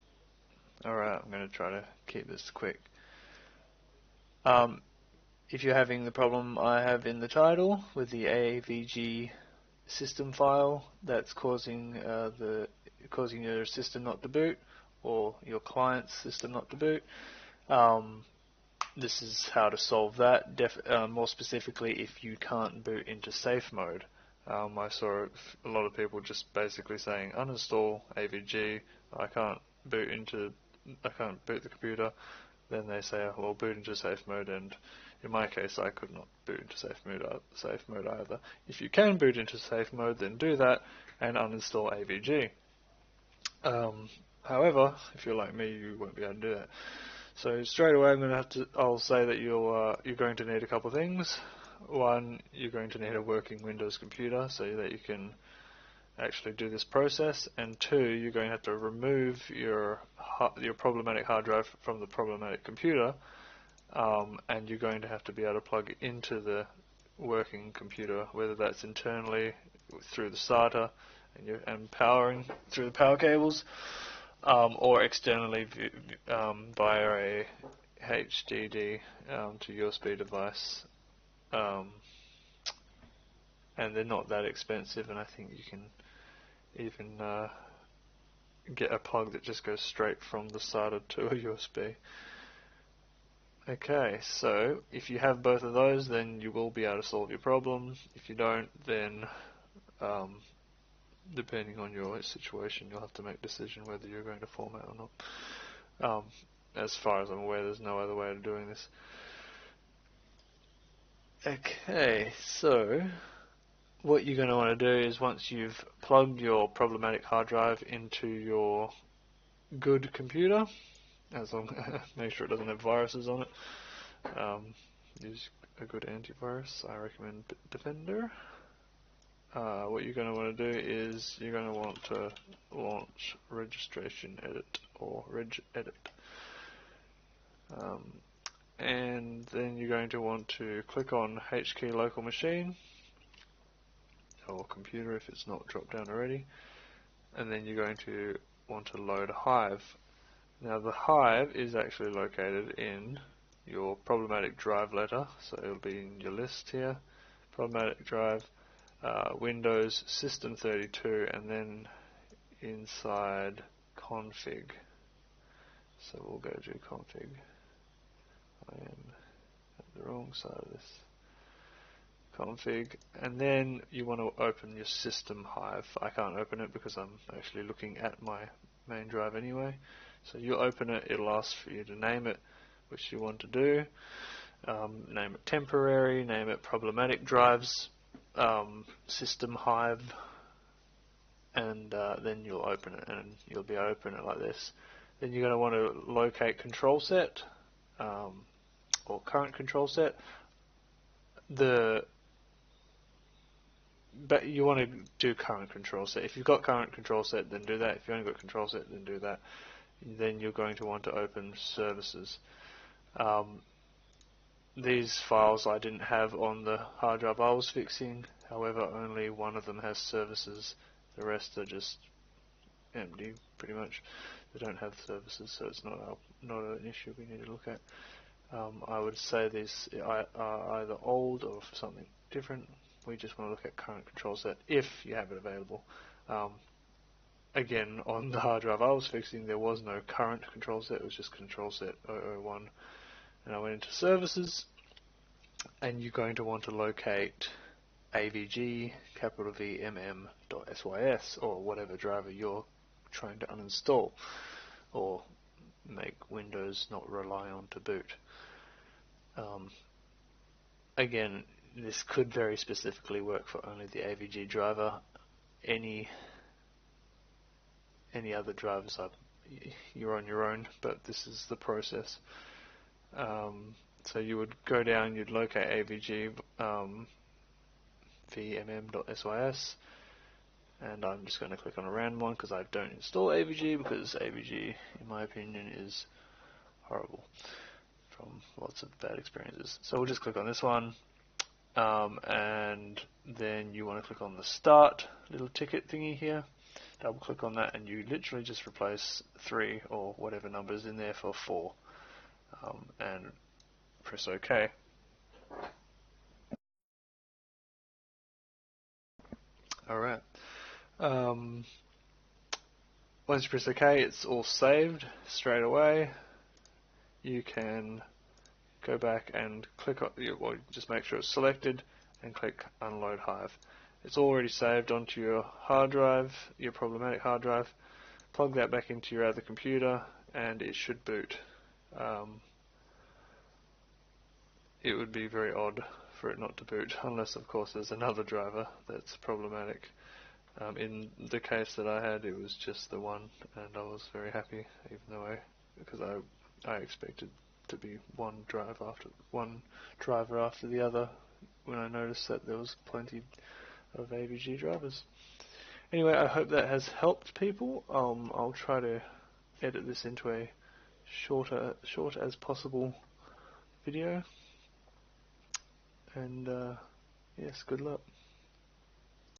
How-to: avgVmm.sys BSOD "corrupt or missing system driver". (Update 2022 Check Description)
Update 2022: you can use bootable media like Hiren Boot CD or USB to boot up the computer on that media and access the registry files on the bad comp. (without removing the HDD from the comp).
Thanks to those in the comment section for alerting me to this.

NOTE: I click on ALG at 4:40, RANDOMLY - Do not edit that one (unless by some coincidence that one is the culprit for you..) I used it at random as I didn't have the avgVmm.sys file that you are guided to click on.